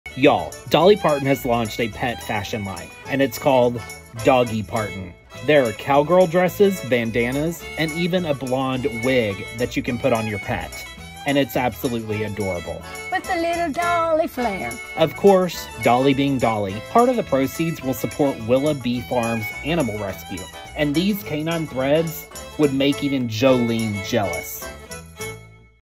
'Doggy Parton' is Dolly Parton's Pet Fashion Line
Model your dog after America's favorite country singer Dolly Parton with..."Doggy Parton."